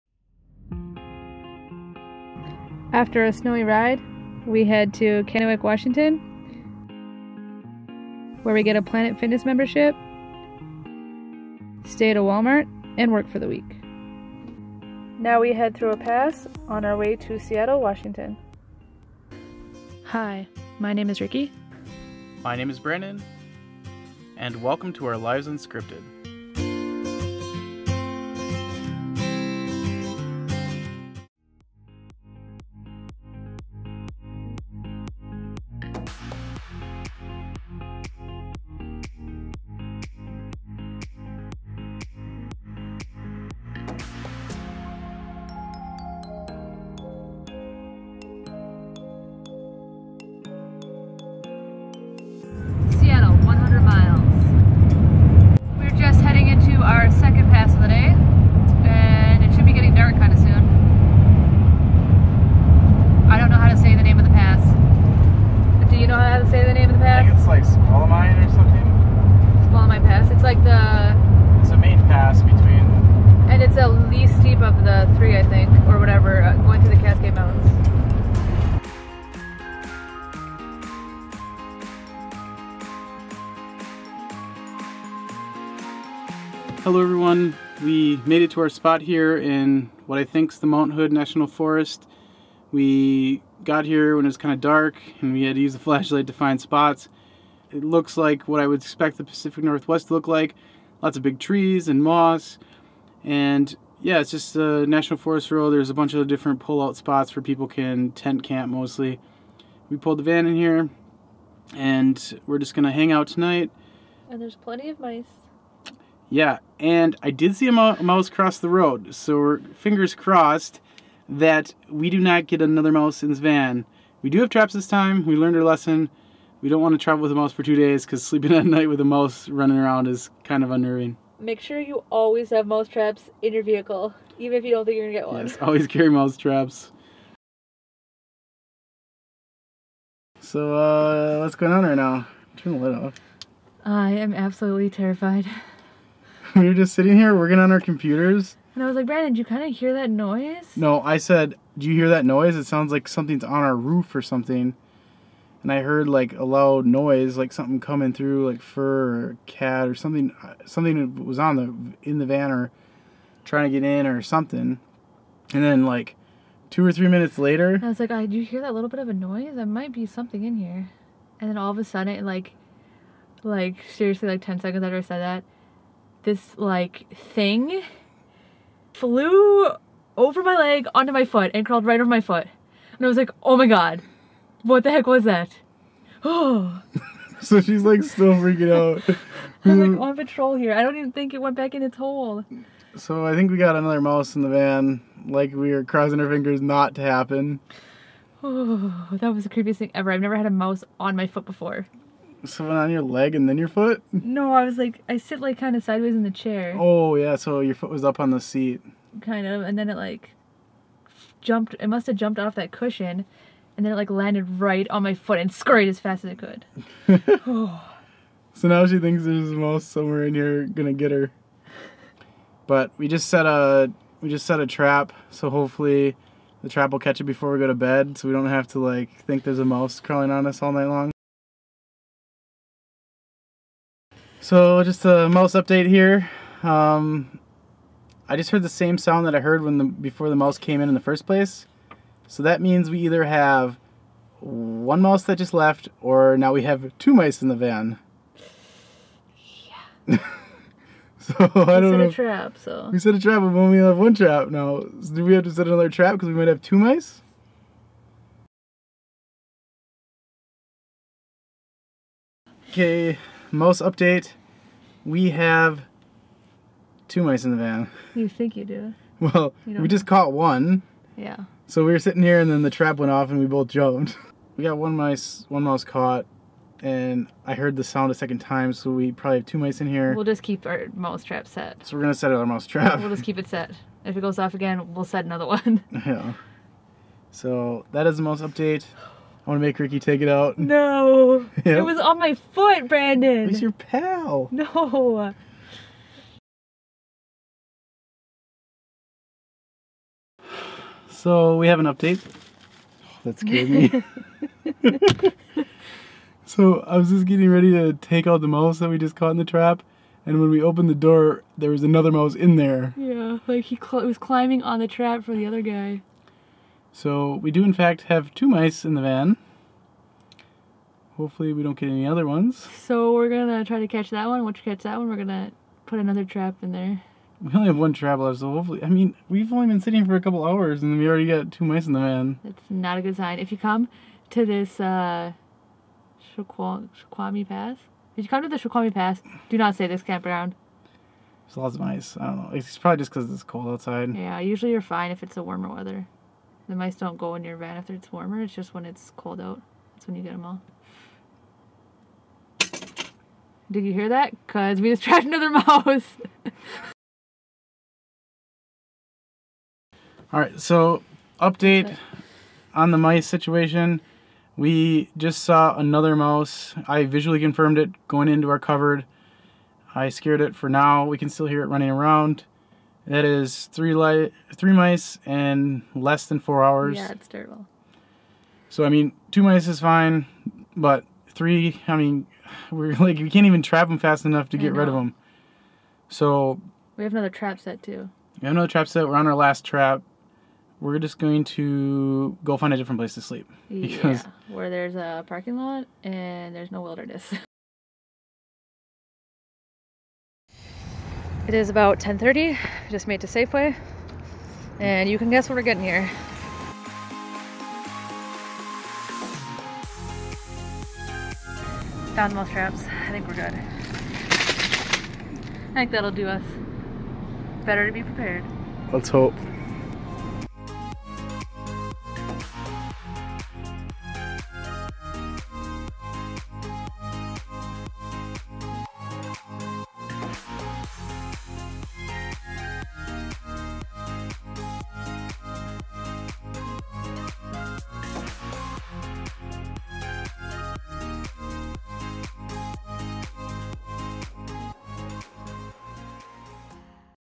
Van Life Vlog : Mouse Invasion (The Downside of Van Life)
Today on, Lives Unscripted, we spend a week in Kenwick, Washington then head through the Snoqualmie Pass to Seattle. But, before we get there we run into a mouse invasion problem.